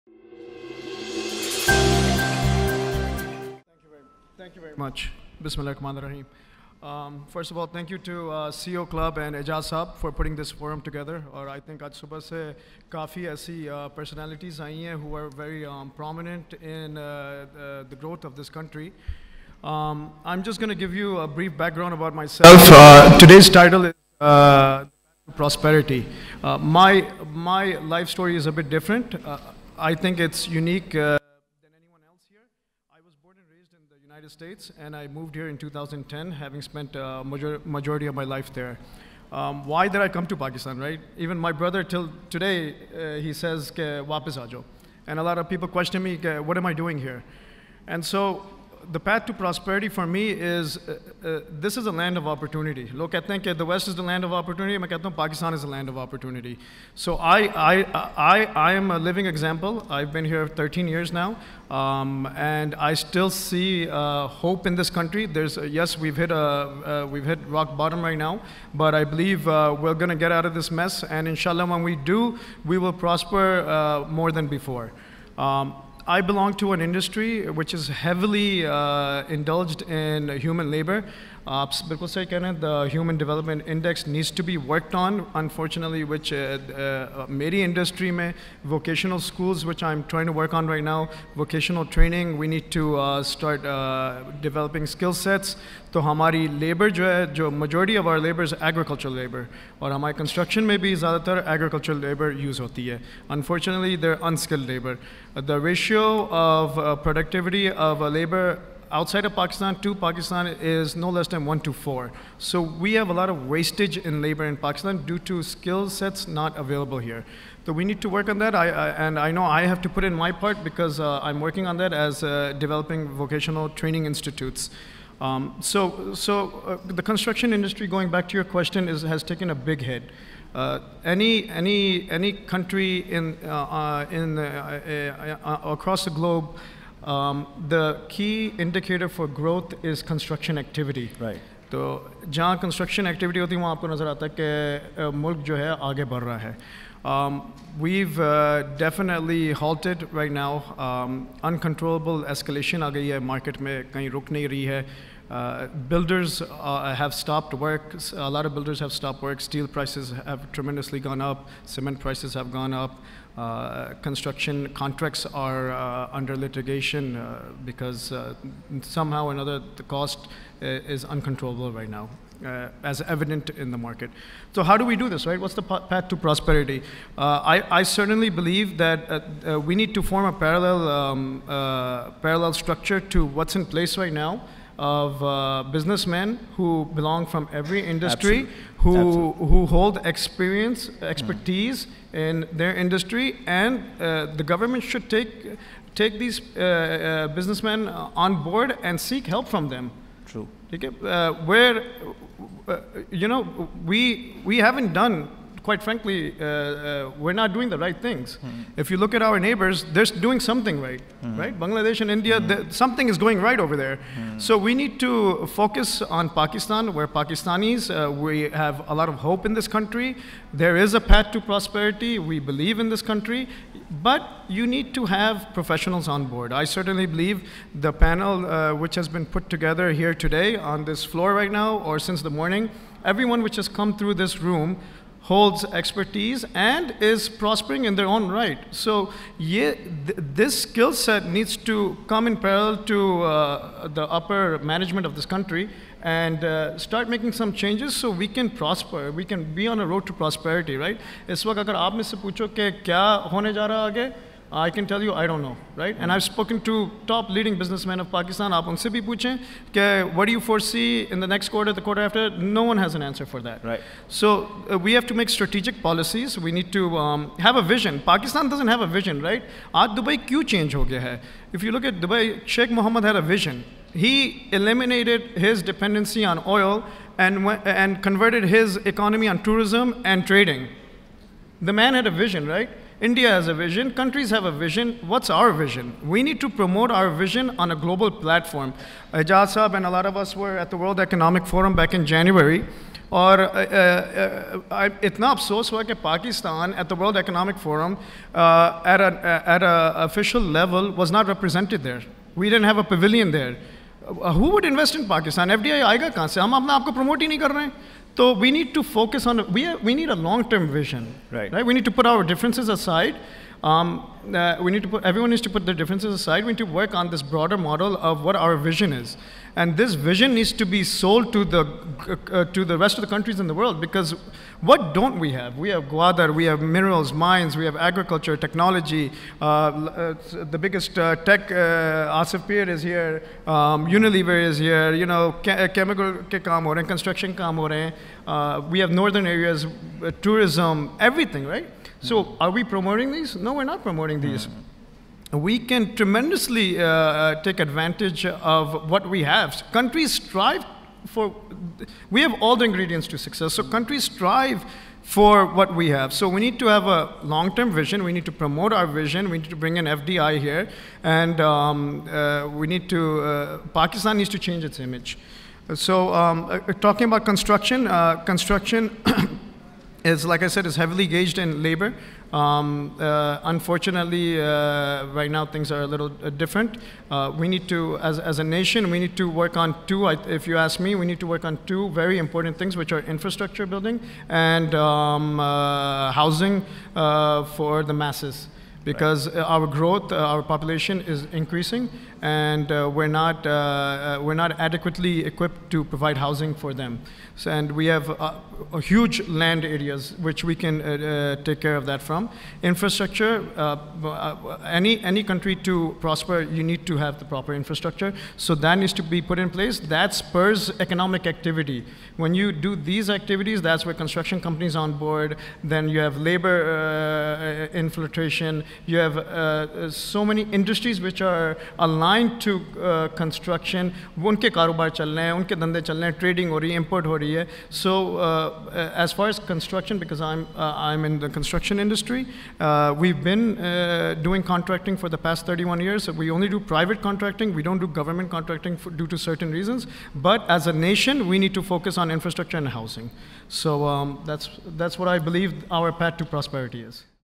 Speech by Mr. Baber Siddiqui at AirLink CEO Summit Karachi 2023!
Watch the exclusive address by Mr. Baber Siddiqui (CEO, Paragon Constructors Pvt. Ltd & President, YPO Capital Pakistan) at AirLink CEO Summit Karachi, the Launch of the Best-Selling Book “100 CEOs & Companies of Pakistan” & CEOs' Leadership Award Ceremony held on 16th March 2023 at Marriott Hotel, Karachi.